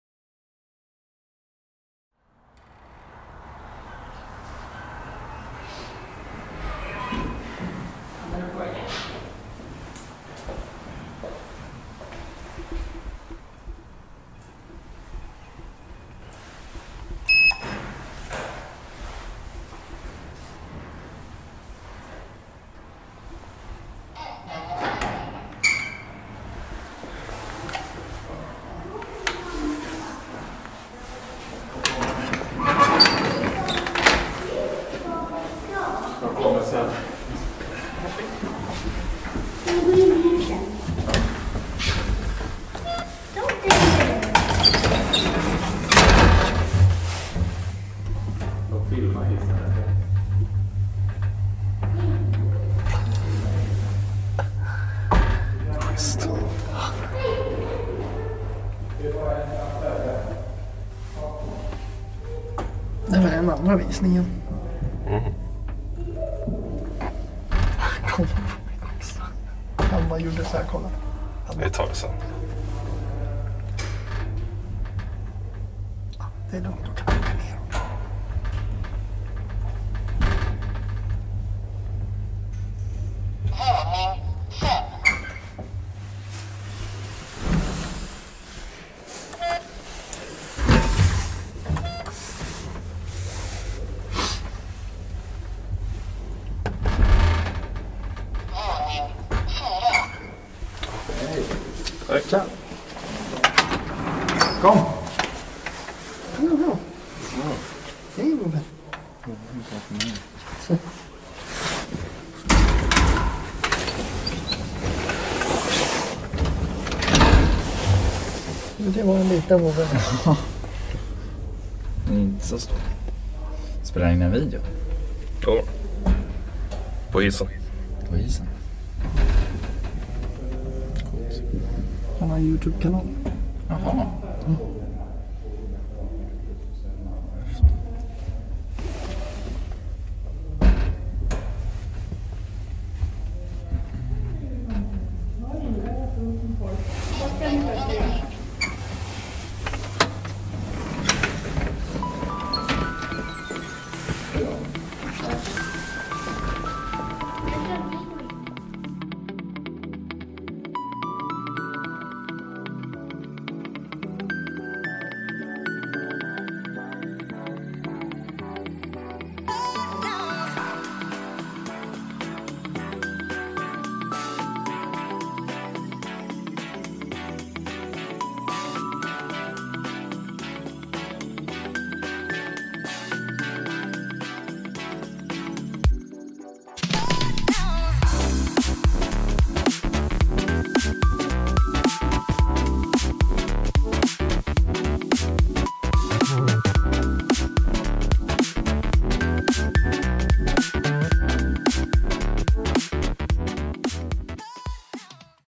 1905 Graham Brothers (MB: ManKan) Traction Elevator @ Hornsgatan 51, Stockholm, Sweden
ELEVATOR INFORMATION!

Current modernizer/Senaste modernisering av: ManKan Hiss AB
Latest modernization year/Senast moderniserat år: 2015
Serial Number/Tillverkningsnummer:
Floors/Antal våningar: 6 = BV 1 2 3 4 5

This elevator has been modernized three times in 1994, 2005, and 2015!

Location/Adress: Hornsgatan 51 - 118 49 Stockholm (Södermalm) SWEDEN!